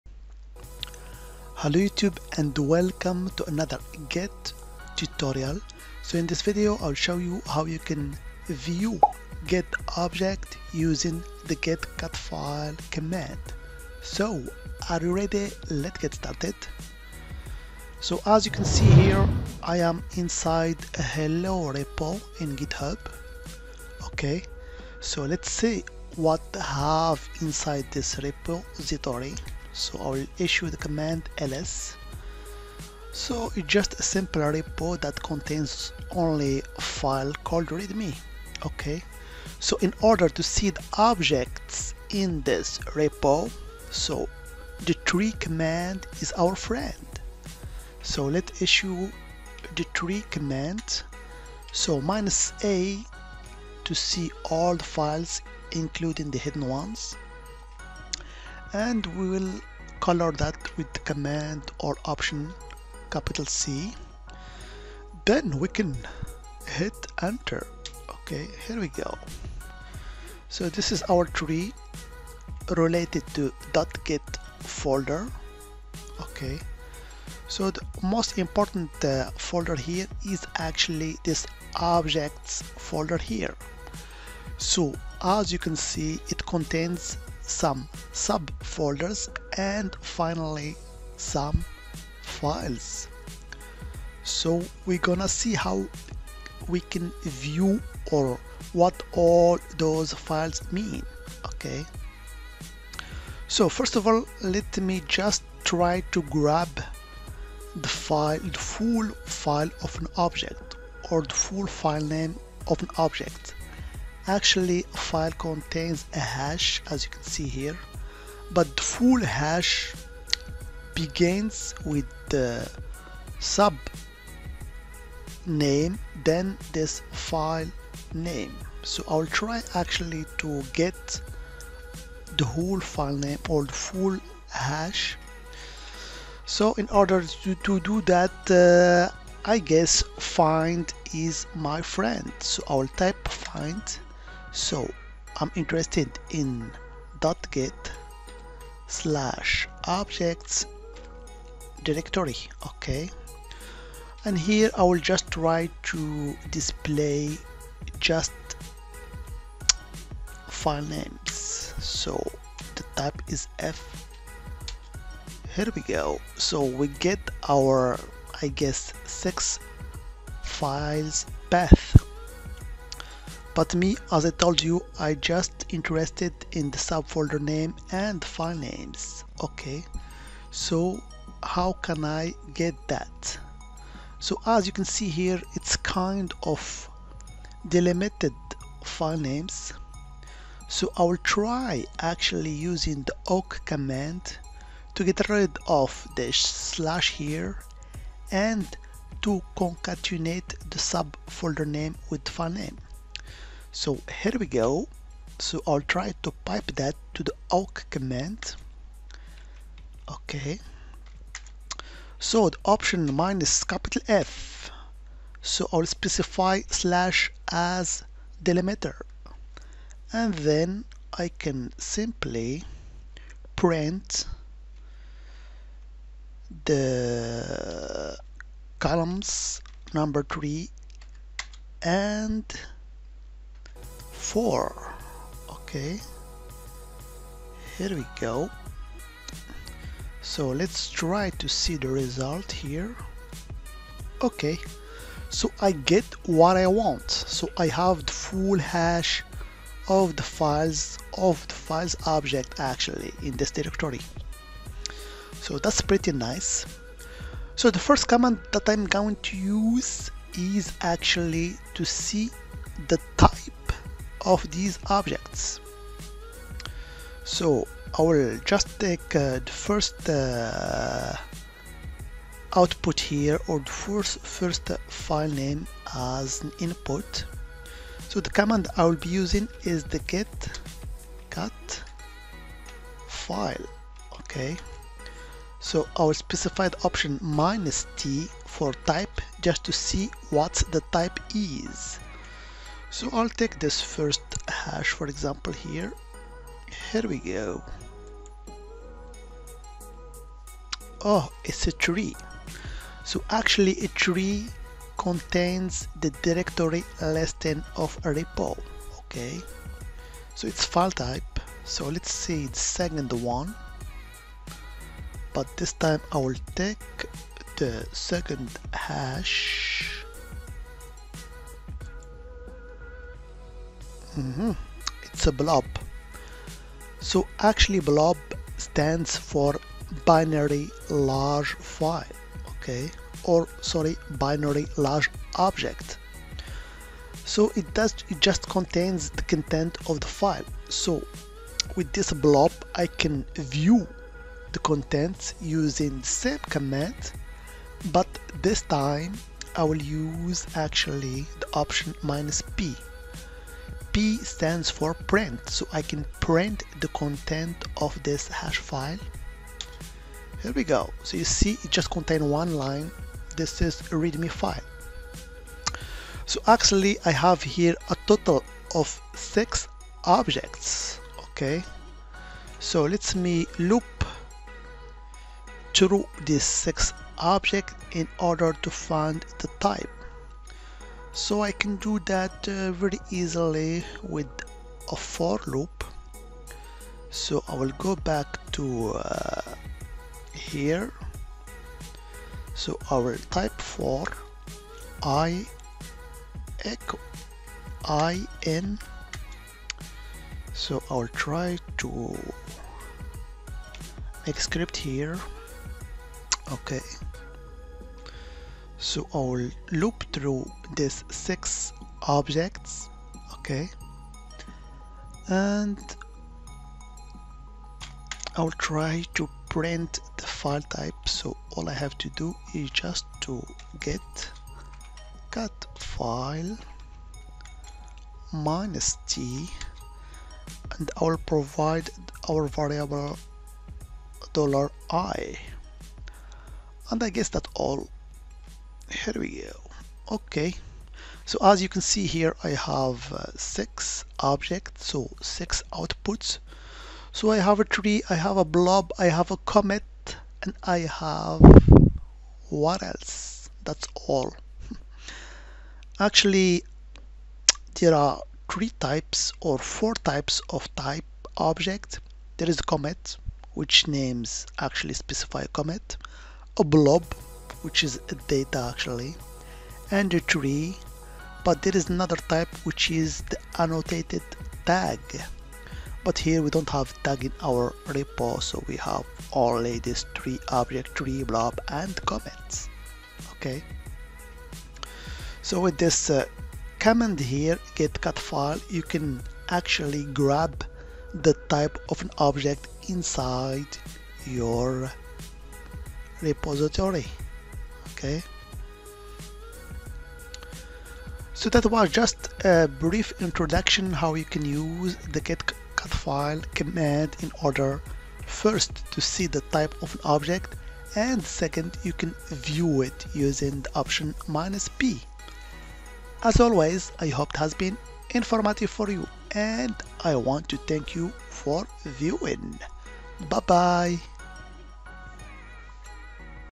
How To View Git Objects using The Git Cat file Command
git-cat-file - Provide content or type and size information for repository objects.